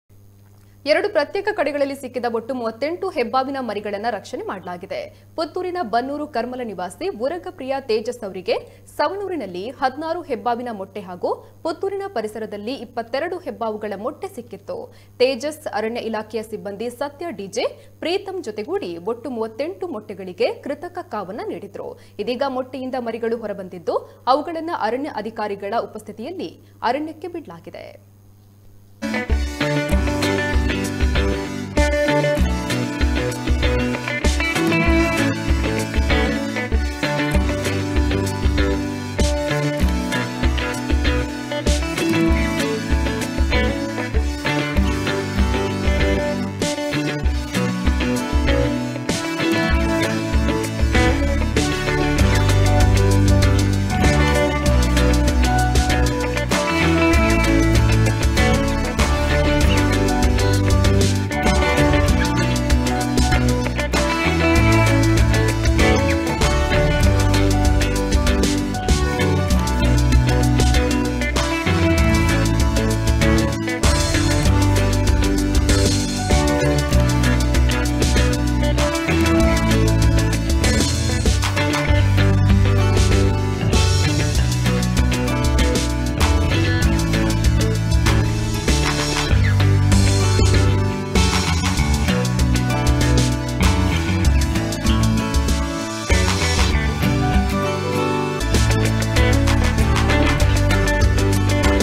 38 ಹೆಬ್ಬಾವಿನ ಮರಿಯನ್ನ ರಕ್ಷಿಸಿದ ಉರಗ ಪ್ರಿಯ ತೇಜಸ್ -ಅರಣ್ಯ ಅಧಿಕಾರಿಗಳ ಉಪಸ್ಧಿತಿಯಲ್ಲಿ ಅರಣ್ಯಕ್ಕೆ ಮರಿಗಳ ಸೇರ್ಪಡೆ
38 ಹೆಬ್ಬಾವಿನ ಮರಿಯನ್ನ ರಕ್ಷಿಸಿದ ಉರಗ ಪ್ರಿಯ ತೇಜಸ್ - ಅರಣ್ಯ ಅಧಿಕಾರಿಗಳ ಉಪಸ್ಧಿತಿಯಲ್ಲಿ ಅರಣ್ಯಕ್ಕೆ ಮರಿಗಳ ಸೇರ್ಪಡೆ – ಕಹಳೆ ನ್ಯೂಸ್